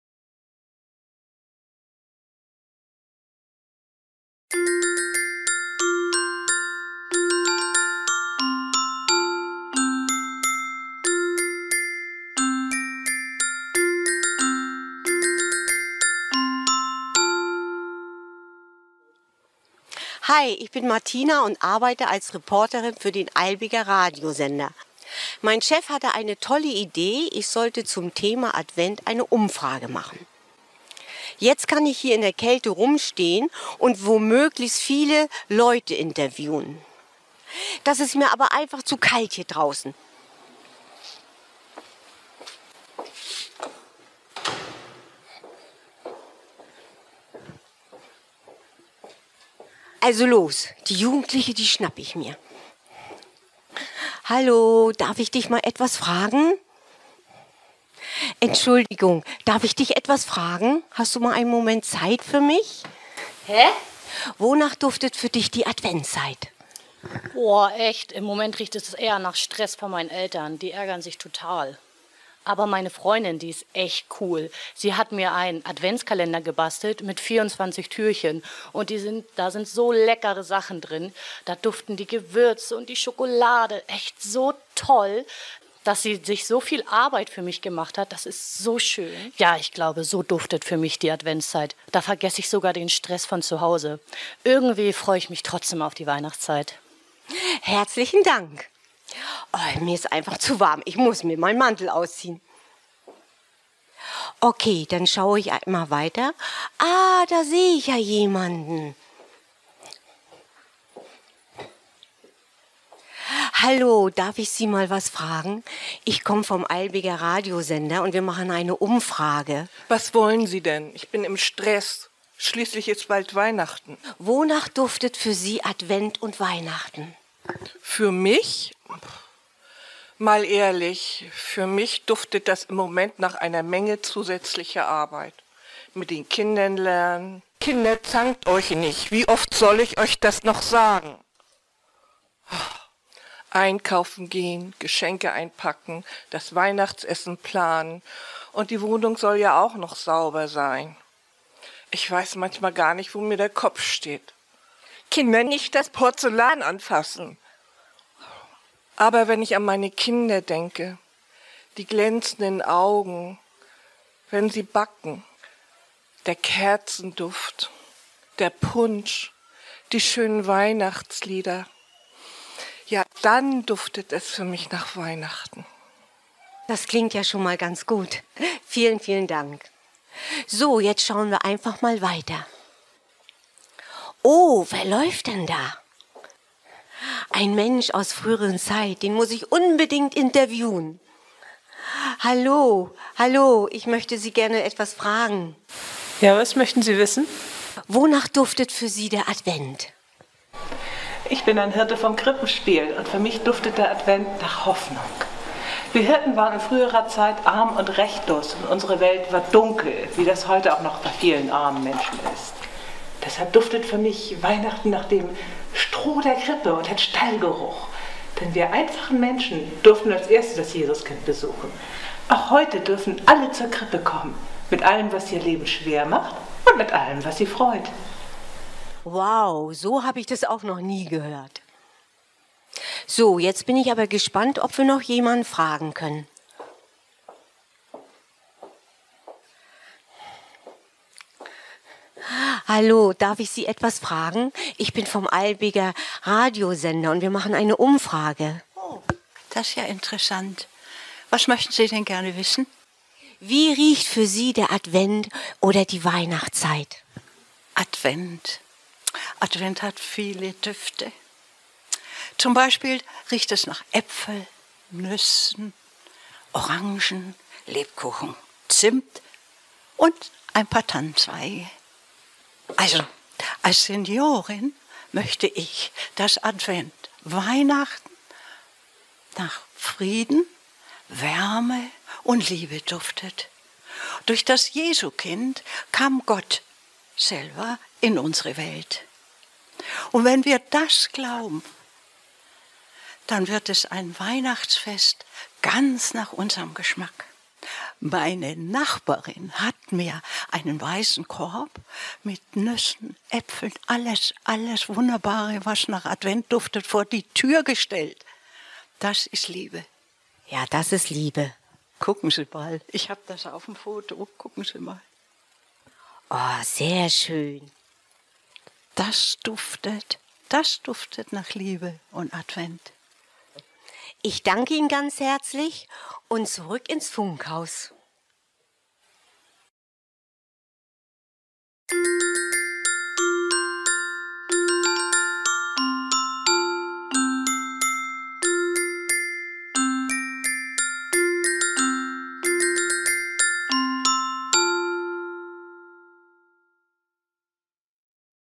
3. Advent - "Wonach duftet Weihnachten"?
Die Theatergruppe der Gemeinde präsentiert "Wonach duftet Weihnachten?"

mit freundlicher Genehmigung der Autorin Martina Liebhäuser - Haggenmüler

Video: Gerd + Idis Eisentraut
Realisation: Renate Endrulat